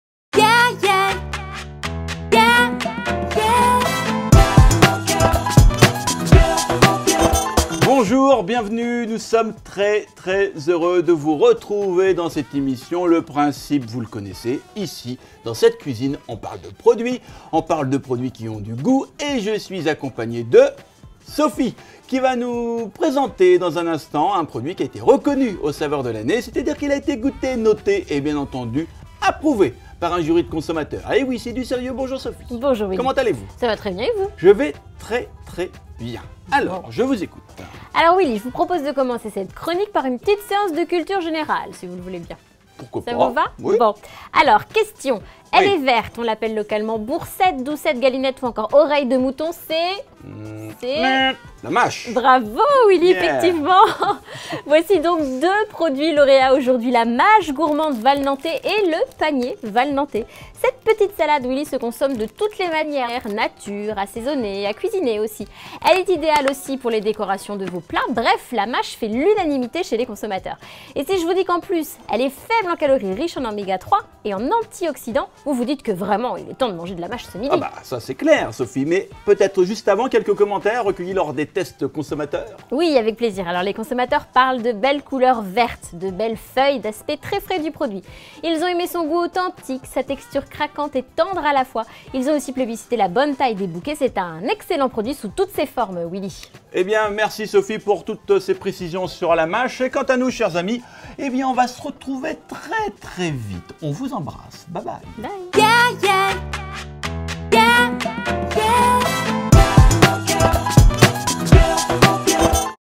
La Mâche Gourmande Valnantais et le Panier de Valnantais
Dans cette vidéo, découvrez une mâche reconnue et appréciée pour son bel aspect et sa couleur verte. Cette petite salade se consomme de toutes les manières différentes : nature, assaisonnée et aussi à cuisiner. Par son bel aspect de bouquet, idéal pour les décorations de vos plats.